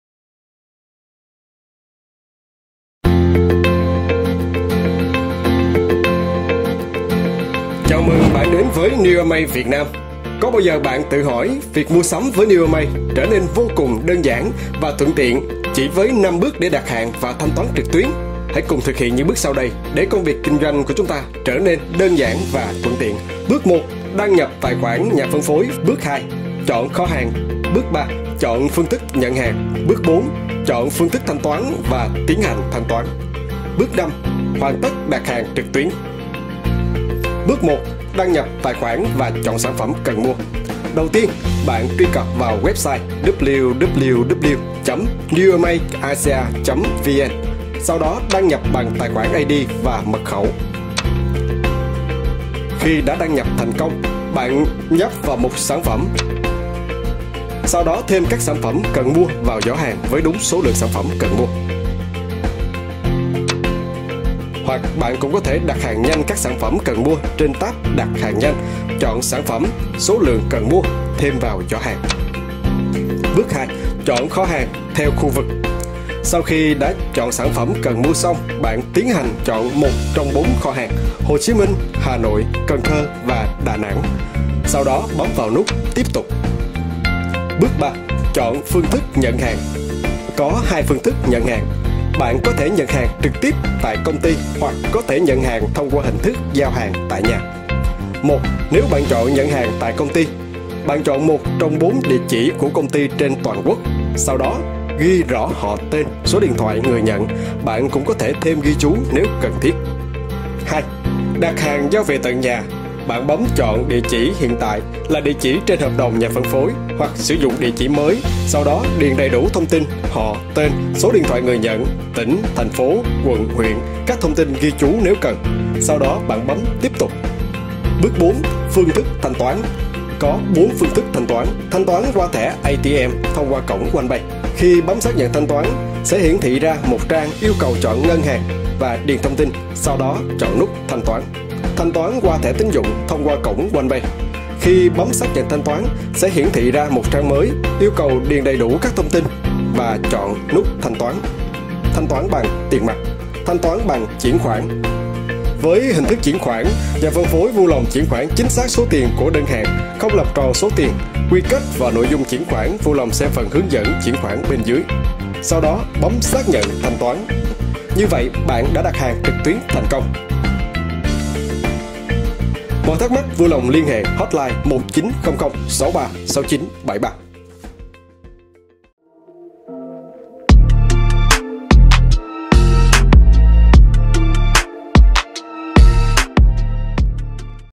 HƯỚNG DẪN ĐẶT HÀNG SẢN PHẨM ALPHA LIPID LIFELINE ONLINE
CÁC BƯỚC ĐẶT HÀNG ONLINE
-Đặt hàng Online
-Đặt hàng online tại nhà, vận chuyển & giao nhận đến tay người tiêu dùng